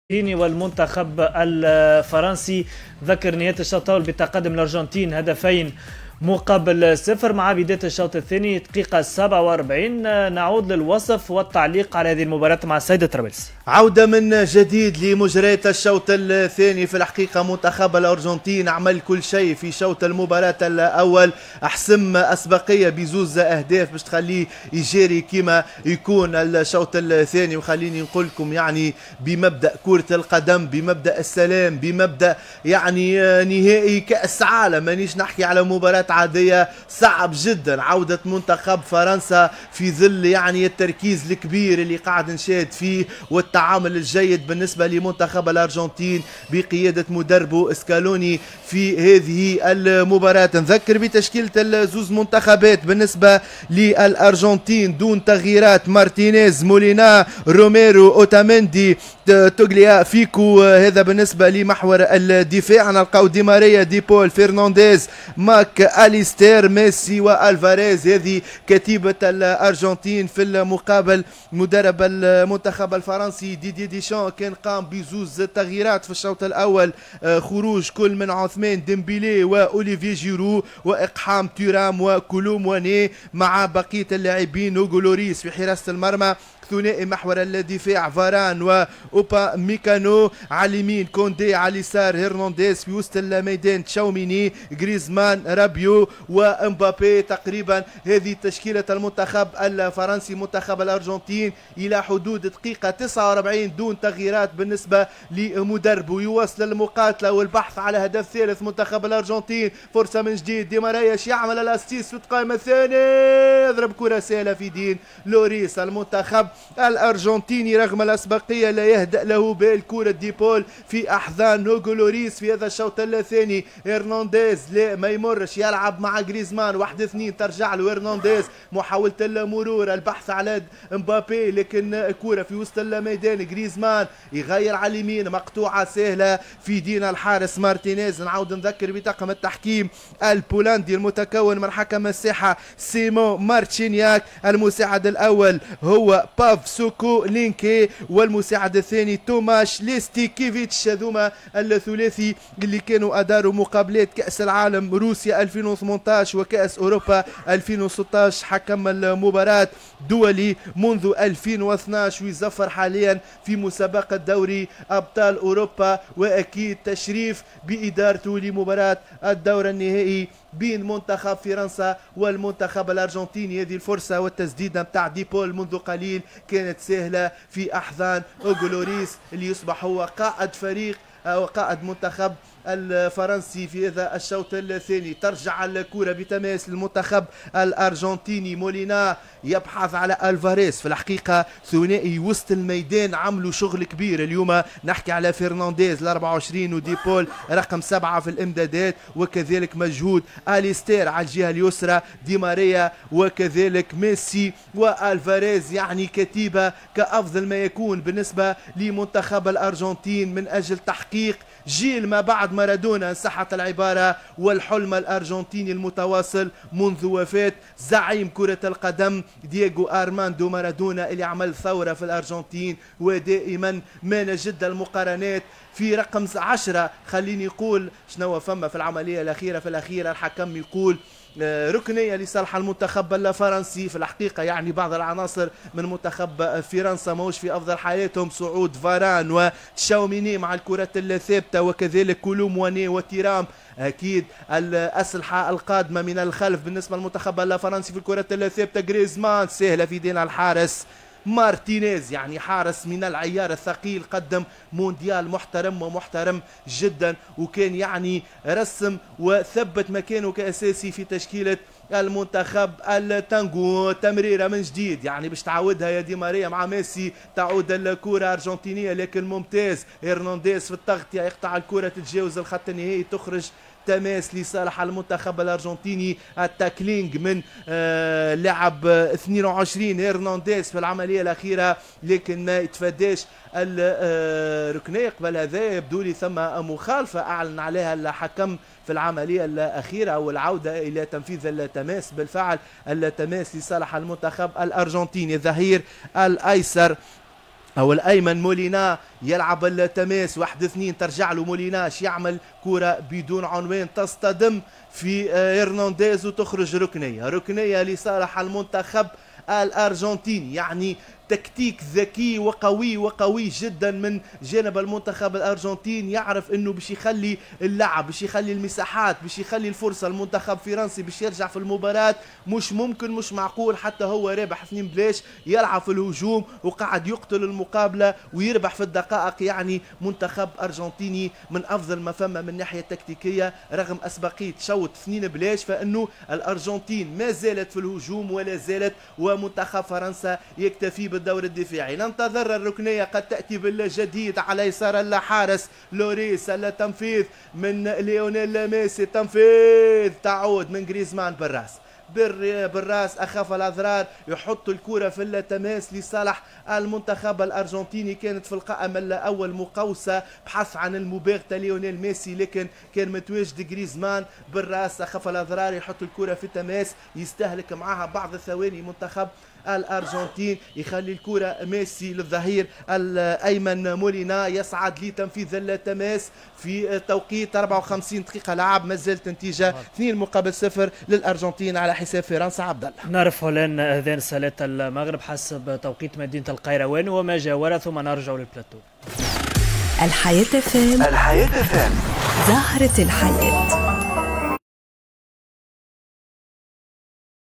ستاد الحياة: نقل مباشر للقاء الدور النهائي بين : المنتخب الفرنسي و المنتخب الأرجنتيني
نقل مباشر للقاء الدور النهائي بين : المنتخب الفرنسي و المنتخب الأرجنتيني